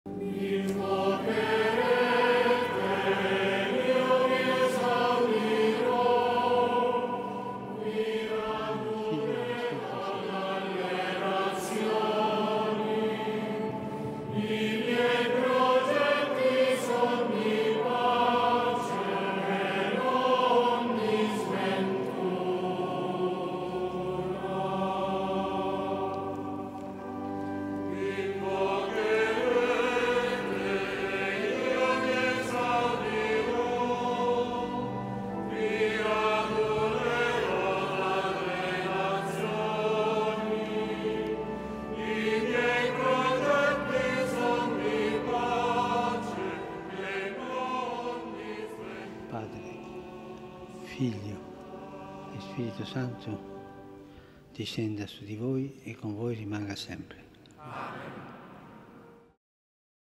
14 November 2021 Holy Mass World Day of the Poor Pope Francis - Videonews
From Saint Peter's Basilica, Holy Mass presided over by Pope Francis to mark the World Day of the Poor